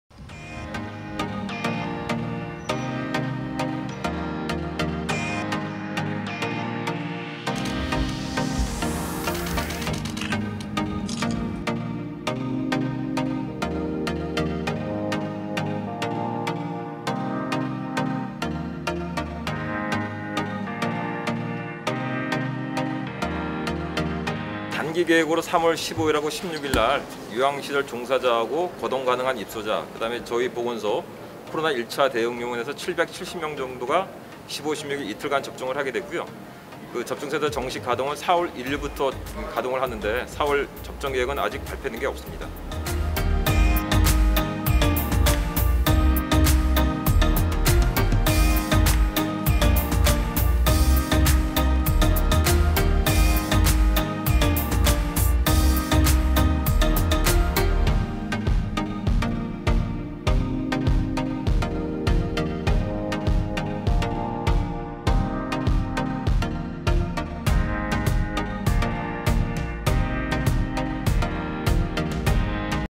춘천 봄내체육관서 코로나 백신접종 시뮬레이션 실시
춘천시가 10일 봄내체육관에서 코로나19 백신 접종에 앞서 시뮬레이션을 실시했다. 15~16일 요양시설종사자·보건소 직원 등 777명 접종 예정이며
4월부터 춘천시민 접종이 이뤄질 예정이다.